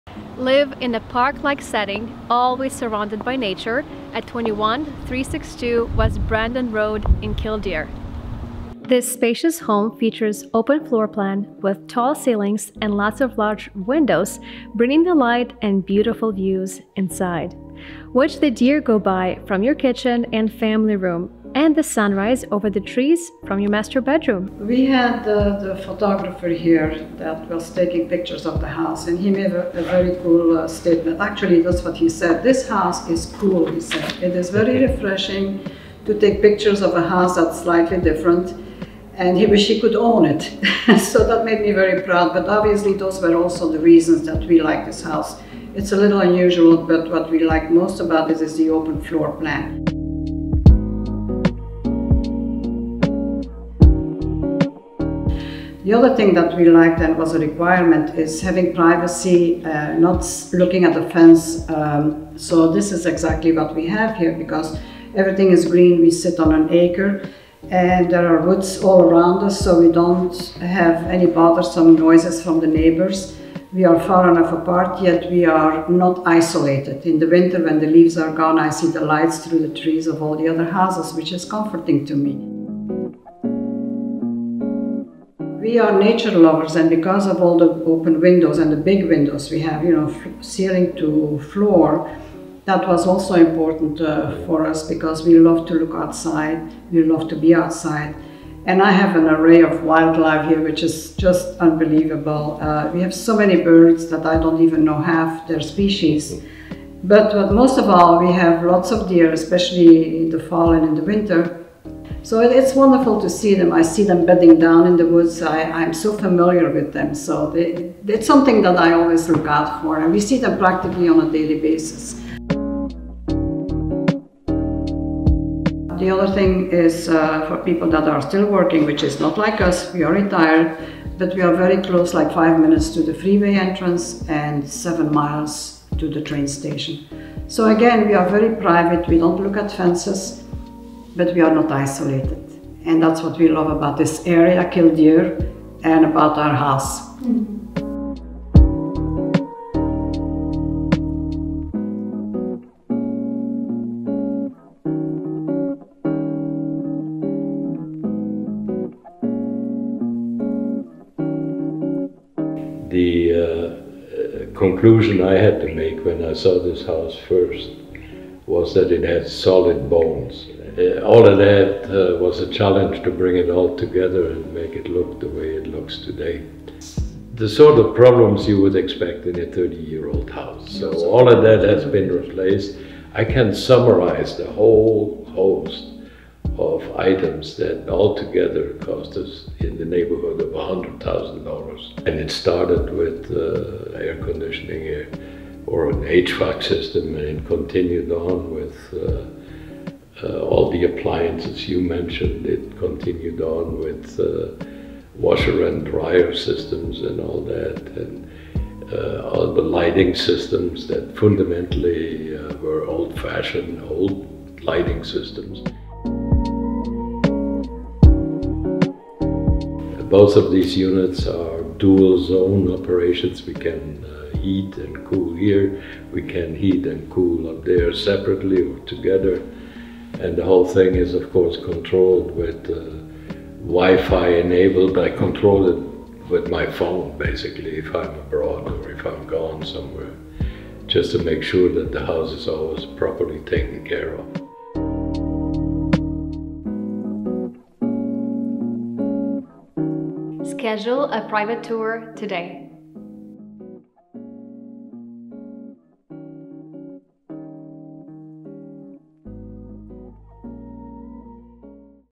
21362 W Brandon Rd in Kildeer. A home in a park-like setting. By Diana Matichyn
Live in a park- like setting, always surrounded by nature!

This spacious home features open floor plan with tall ceilings and lots of large windows bringing the light and beautiful views inside.

Watch the deer go by from your kitchen and family room and the sun rise over the trees from your master bedroom.

The home has been fully upgraded, the owners have taken great pride and a large financial investment with about $100,000 in improvements to make this property move in ready. From new hardwood floors, updated kitchen with advanced technology LG stainless steel appliances, to new HVAC; mechanical & electrical systems with Wi-Fi controls providing dependable & most energy-effective operation. Also new humidifier, water heater, water softener, well pump, generator, and new roof.

Enjoy the picturesque views from the large wrap around deck and a serene, private setting all around.

The home is conveniently located in a quiet location in the dead-end street yet still close to shopping at the Deer Park mall, 3 miles away to highway entrance and 7 miles to train station.